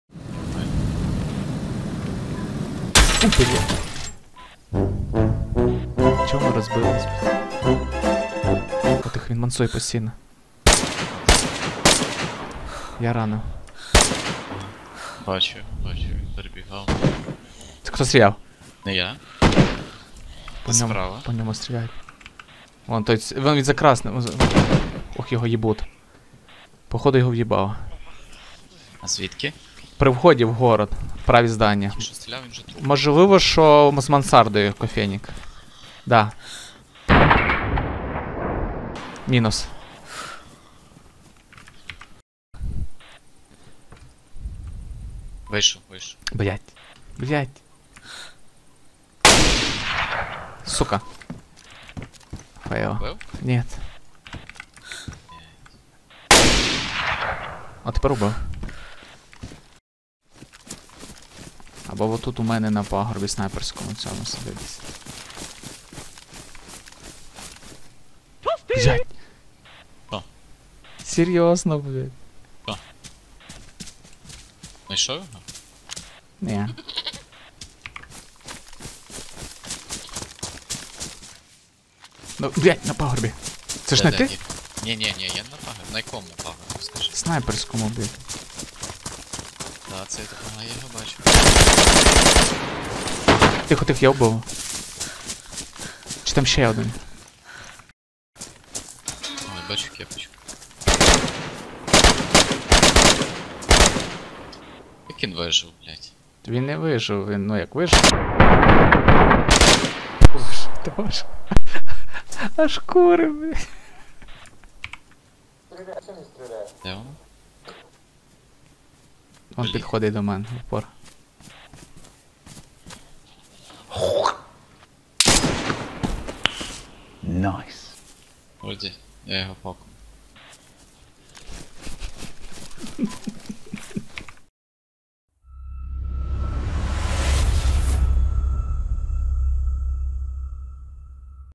last fight(not) before new patch
нарізка деяких моментів перед виходом нового патчу
завдання було пробігати всю карту, розжитися бронею, повернутись на берег.